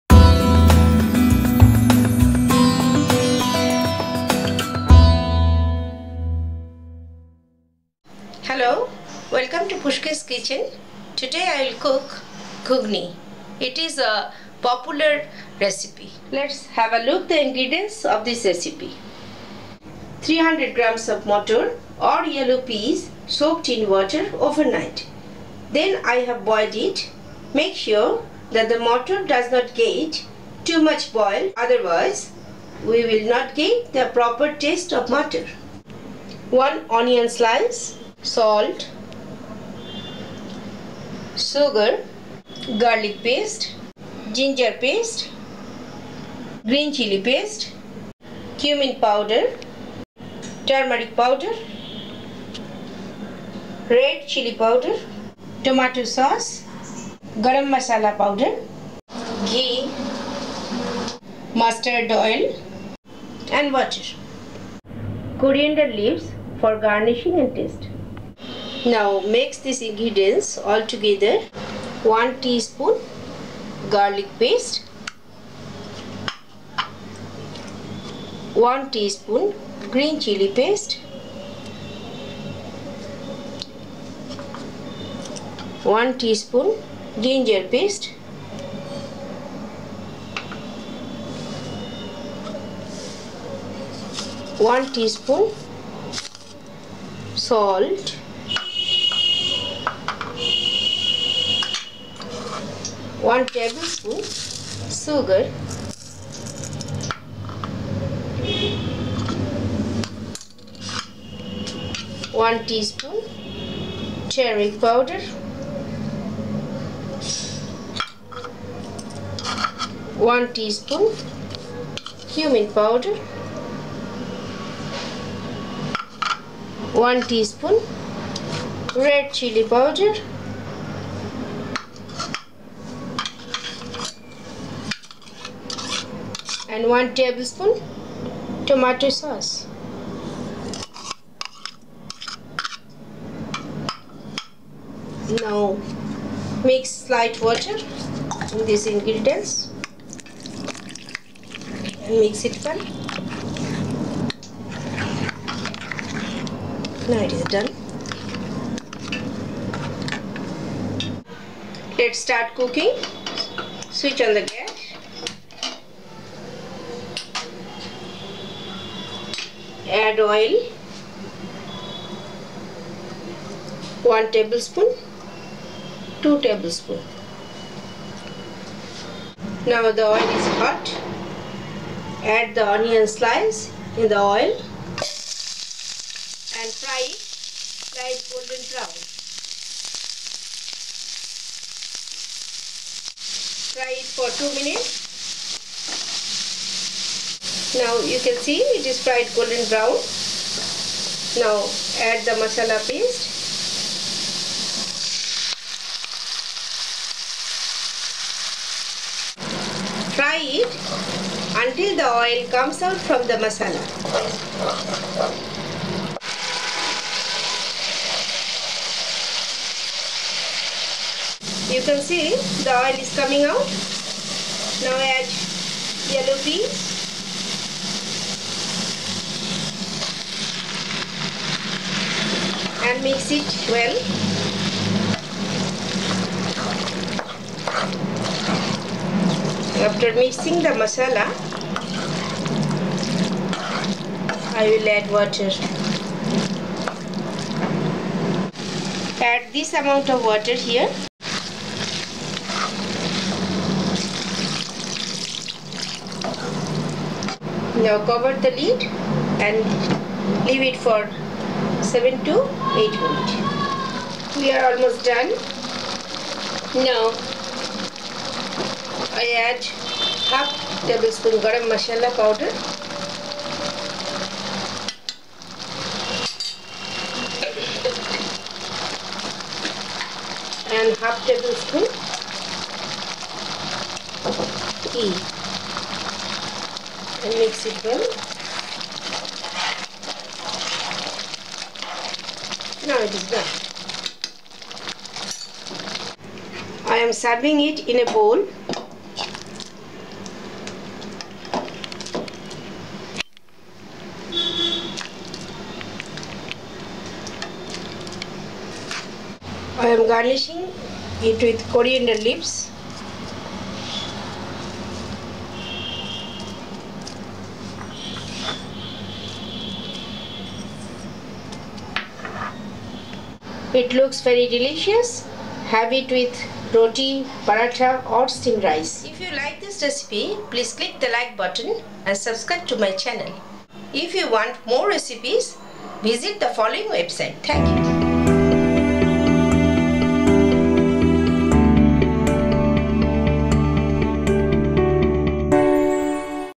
Matar Gughni/Yellow Peas Curry(Bengali/Indian style Gughni recipe)
Gughni is very popular and well known recipe in India.It is very delicious dish.It doesn't take too much time to prepare.Enjoy it with puri,parata,roti or hot steamed rice.Have a wonderful day.